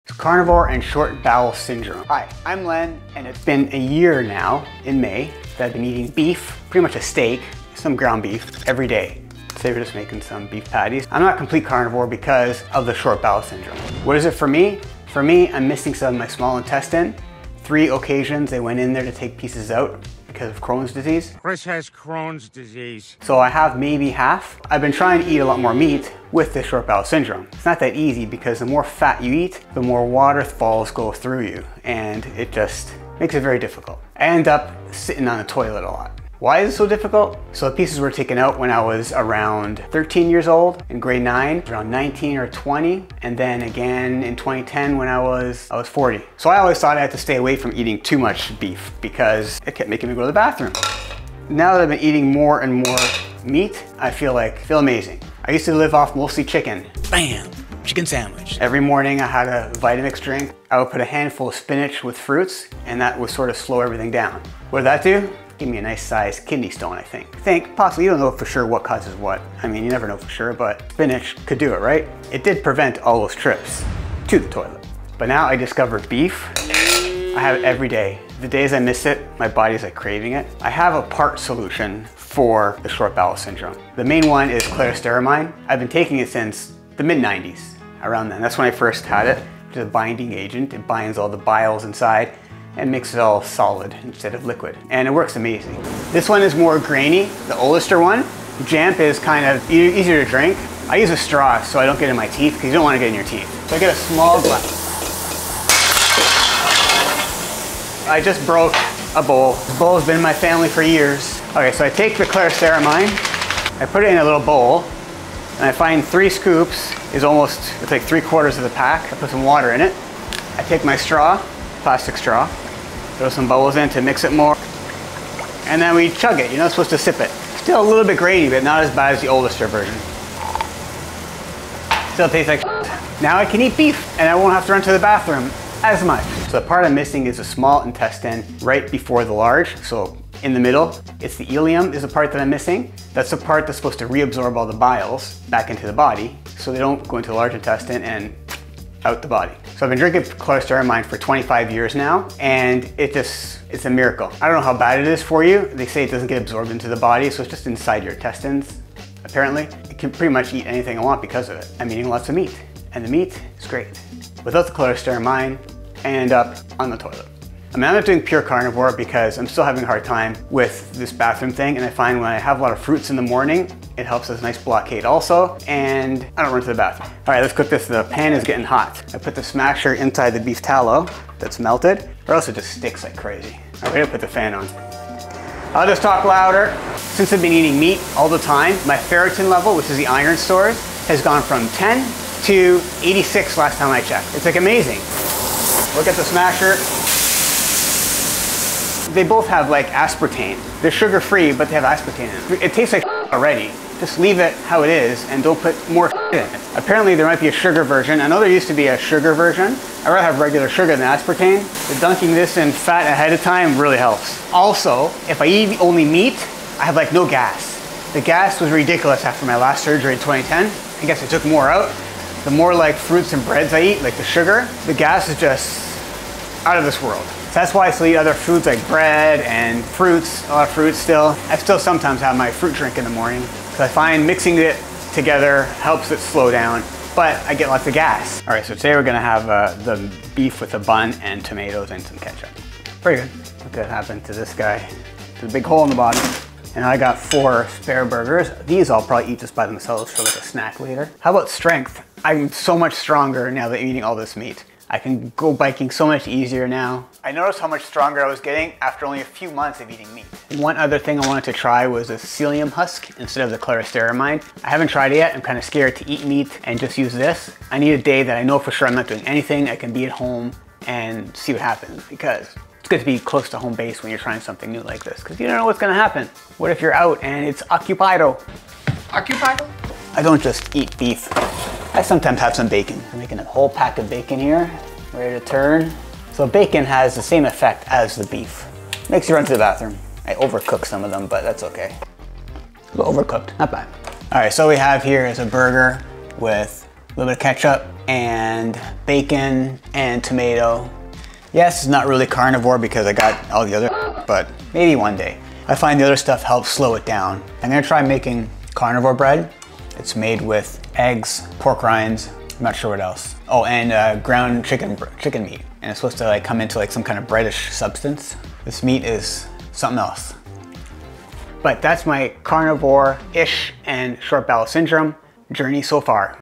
Eating a Carnivore Diet with Short Bowel Syndrome (SBS)
A little info about eating Meat with Short Bowel Syndrome.
Carnivore Diet with SBS